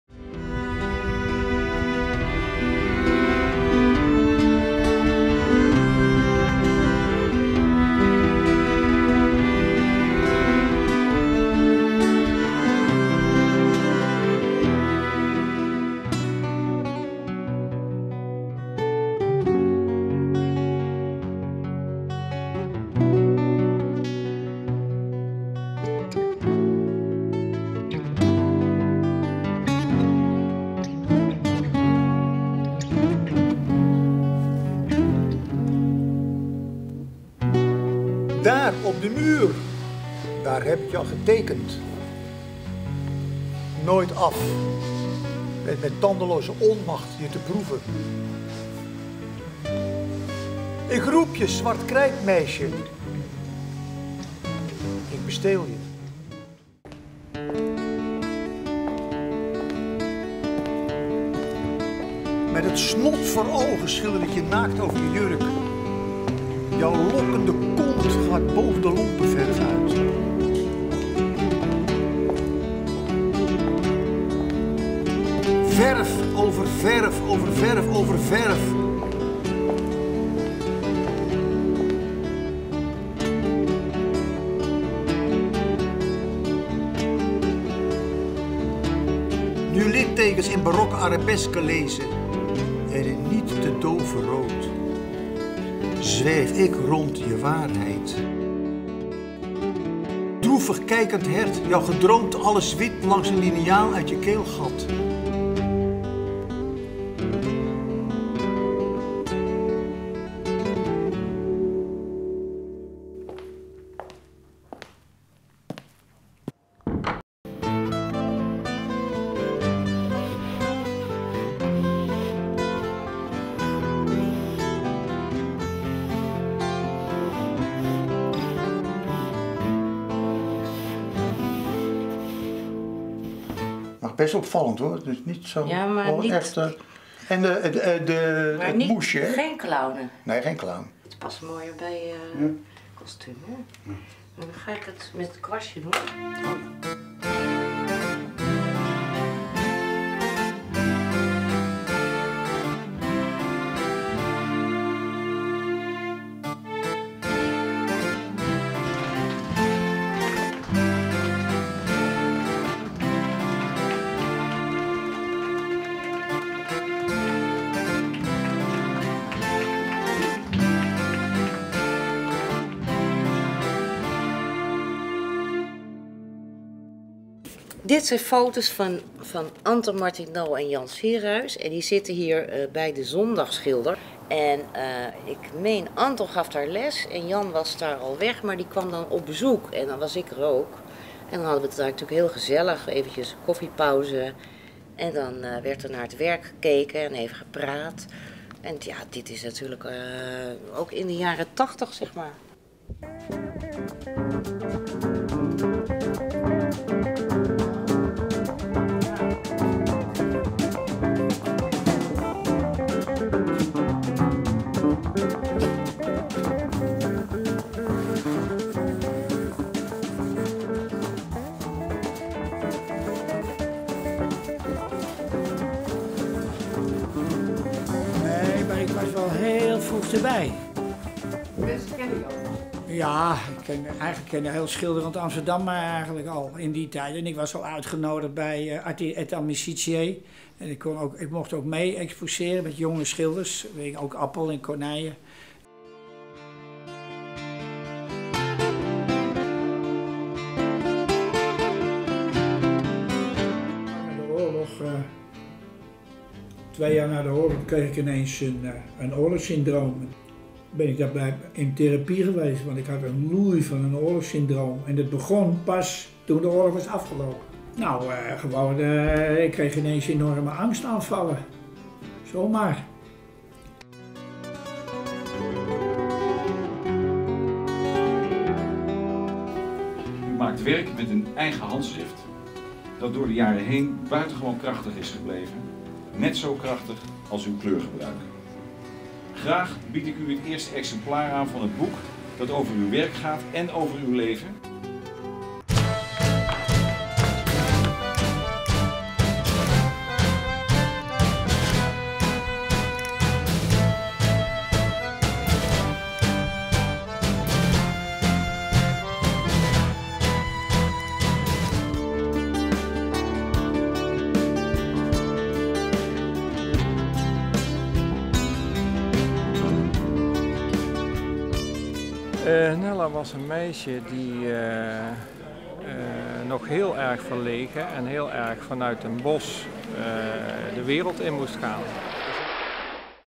muse and the masters t#4A56.mov
Een documentaire over twee kunstschilders,
Jan Sierhuis En Anton Martinau en hun Muse
Nella Tenking.